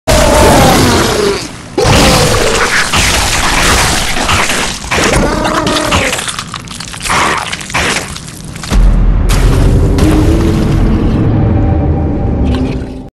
Ayyyy Lmao 8: Street Shark Snacktime
Normally multihit, high damage grabs you cant do much to escape from annoy me, but that just cracks me up.